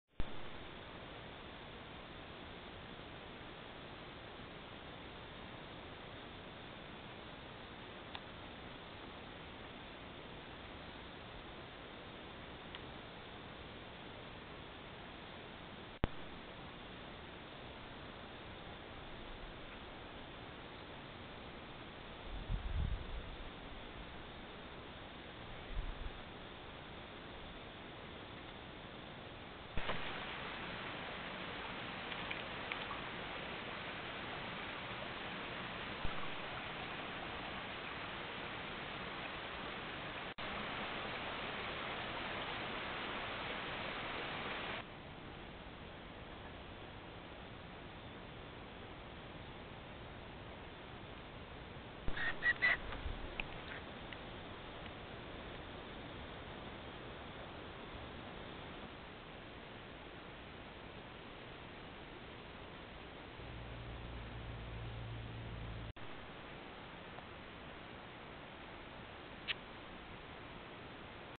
Possum compilation
A few of the best shorts of possums captured at different spots around our plot over the last month. These are brushtail possums, the most common possum in Tasmania and a bit of a bruiser! A couple of the vids look rather like the smaller ringtails but I'm not confident to call it given the not-brilliant trailcam pictures. The brushtails are very cute and blunder around on the ground and in the trees, often leaving a trail of destruction in their wake! Can't help but love 'em though - they are the comedians of the marsupial world.